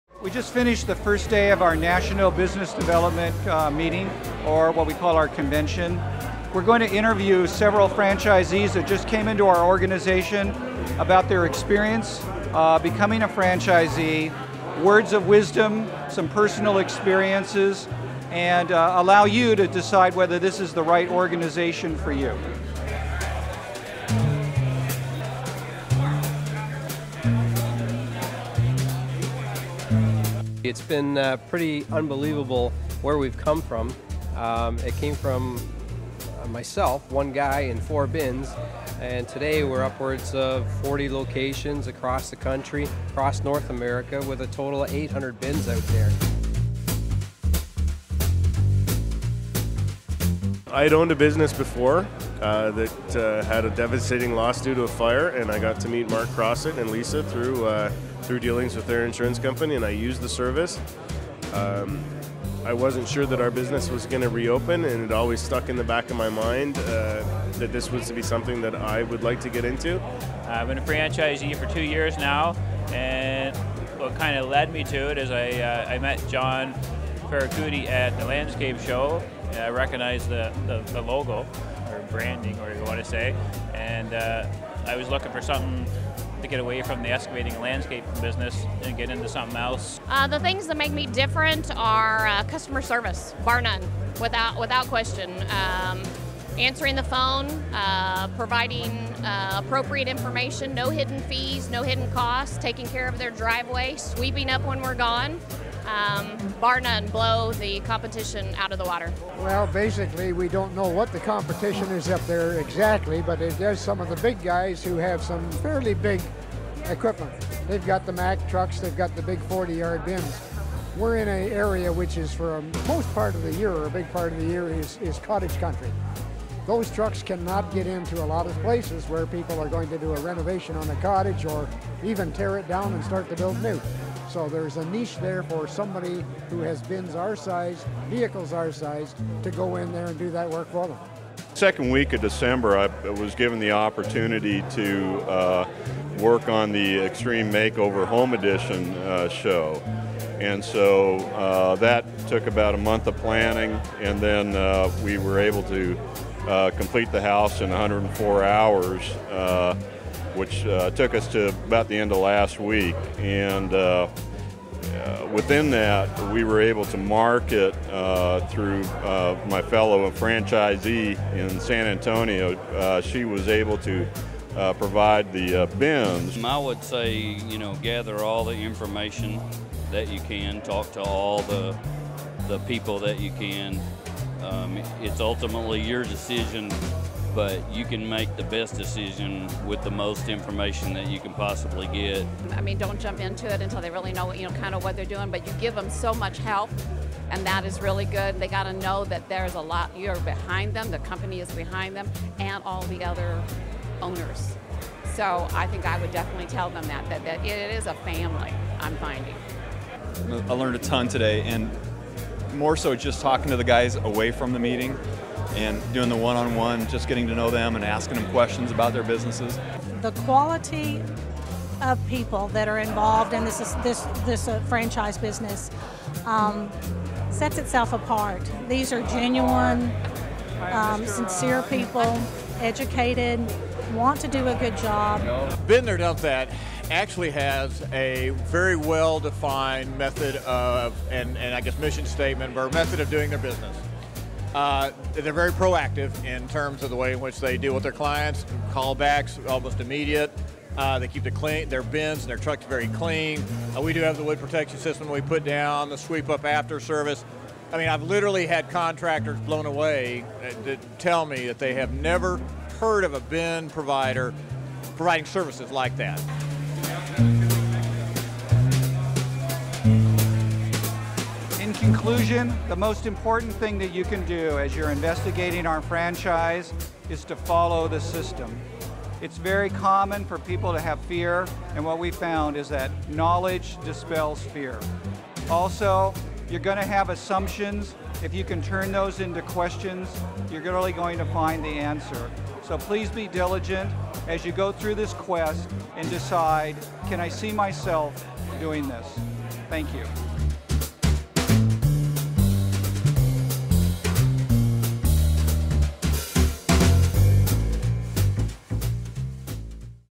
Bin There Dump That - National Business Development Convention
With Bin There Dump That it's a simple six step guided investigation to help you decide if this opportunity, Bin There Dump That, is for you. This is the underlying core of what makes franchising so successful - the system. Using a systems based approach doesn't start when you become a franchise operator, it starts now with your due diligence.